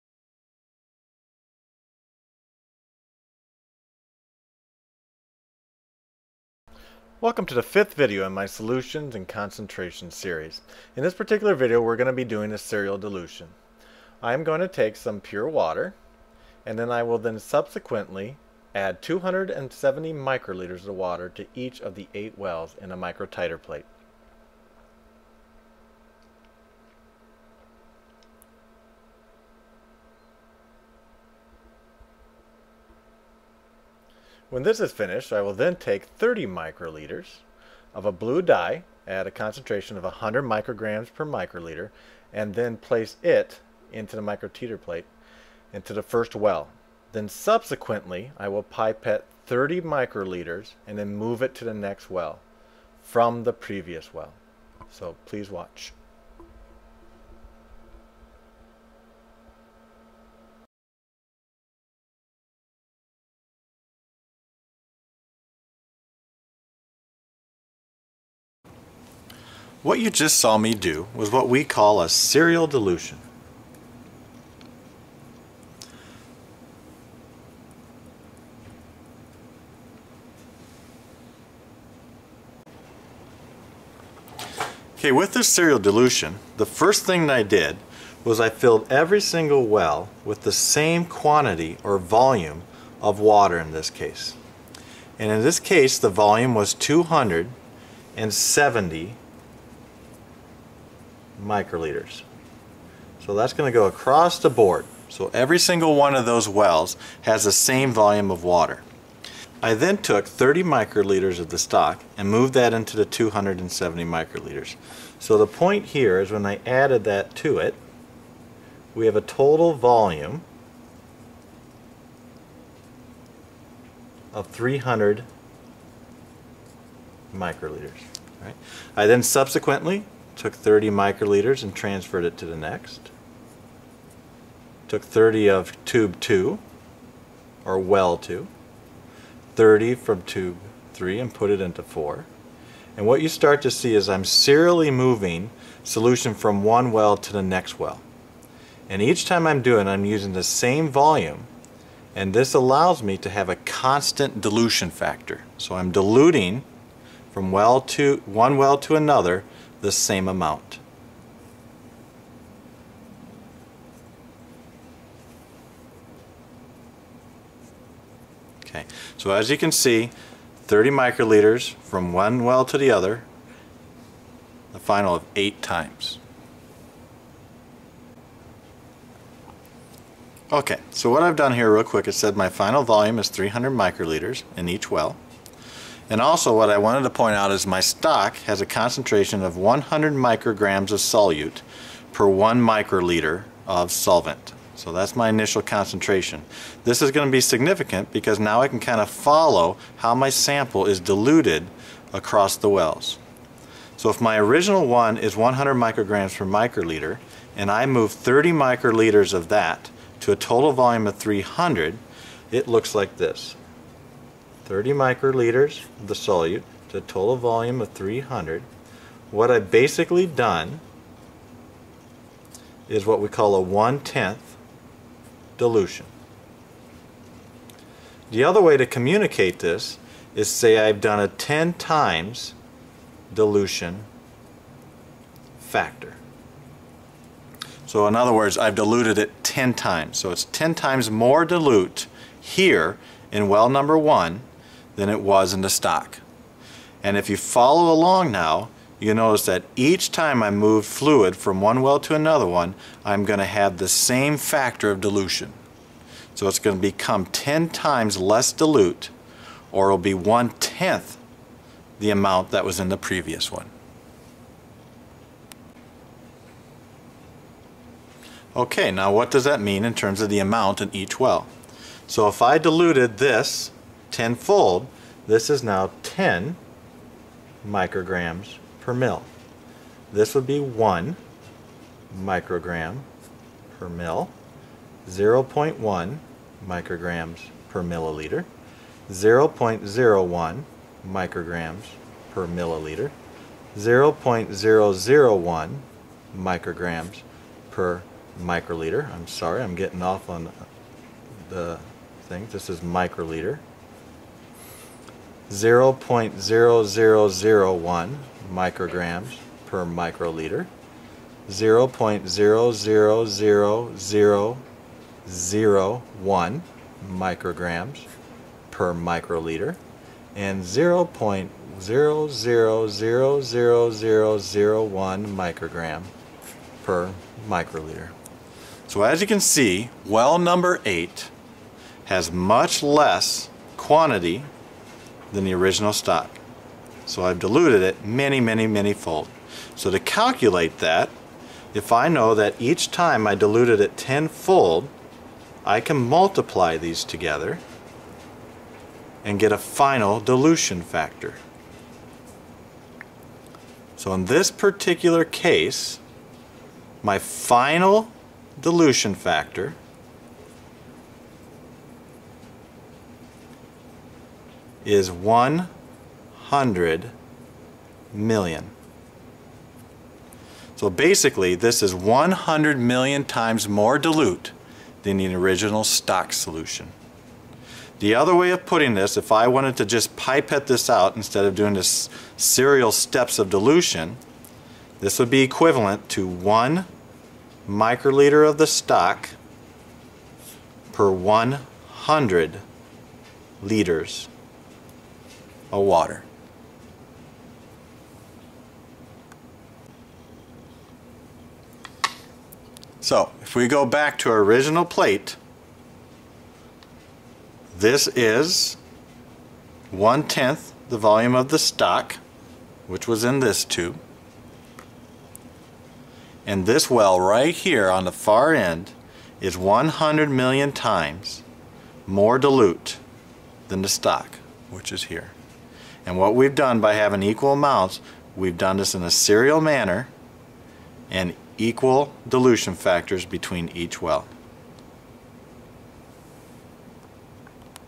Concentrations Part 5 - serial dilution
Fifth video in a series of videos discussing concentration calculations commonly used in a laboratory. More specifically a molecular biology lab. This particular video shows the basics behind setting up and calculating a serial dilution. This procedure can be used to establish a standard curve from a solution of known concentration.